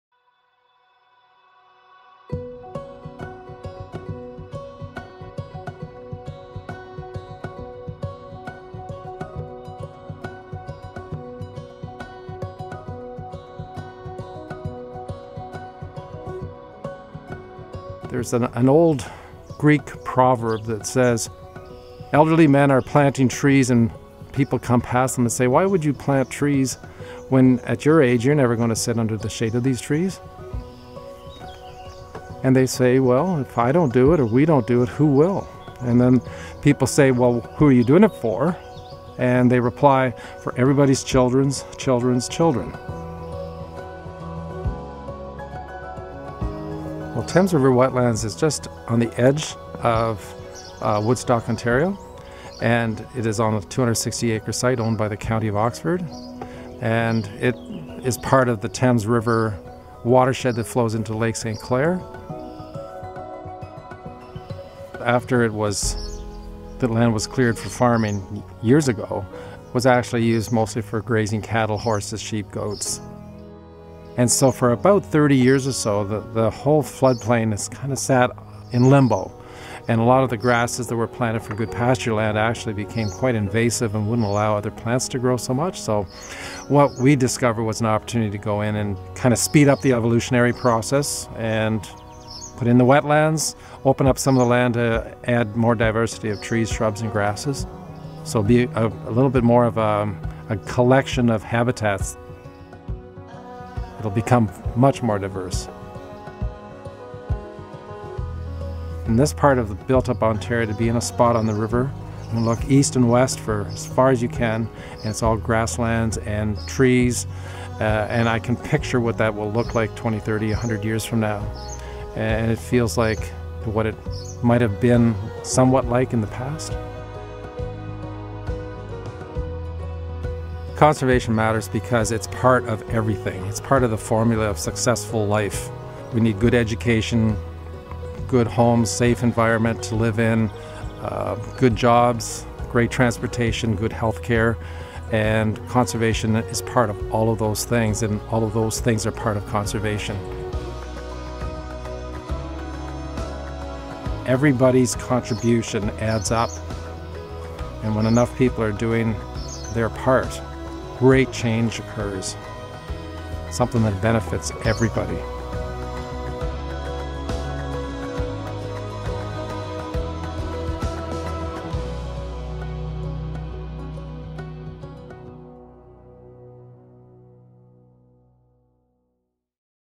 Thames River Wetlands: restoring diversity in southern Ontario
“Conservation matters because it’s part of everything. It’s part of the formula of successful life... Everybody’s contribution adds up. And when enough people are doing their part, great change occurs.” DUC director Phil Holst is a driving force behind habitat restoration at the Thames River Wetlands. His contributions to conservation are changing this area in southern Ontario into a more diverse and welcoming place for waterfowl, songbirds and a variety of other wildlife.